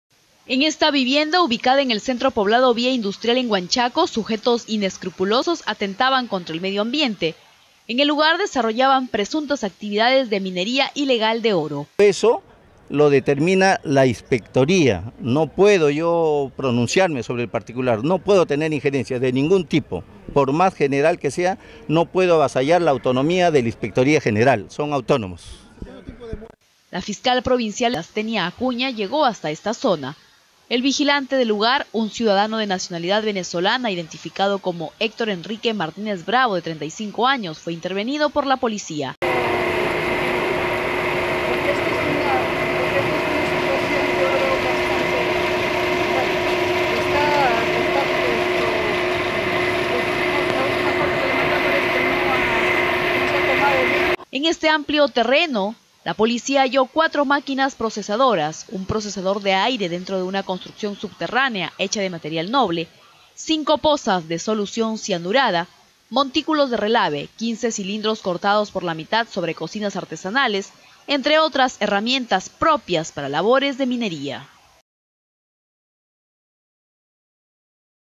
Trujillo: intervienen terreno donde ejercían minería ilegal
La Policía intervino un inmueble en el centro poblado vía industrial en Huanchaco, donde realizaban presuntas actividades de minería ilegal de oro atentando contra el medio ambiente. El vigilante fue intervenido por la policía.

En esta vivienda, ubicada en el centro poblado Vía Industrial en Huanchaco, sujetos inescrupulosos atentaban contra el medio ambiente. En el lugar desarrollaban presuntas actividades de minería ilegal de oro.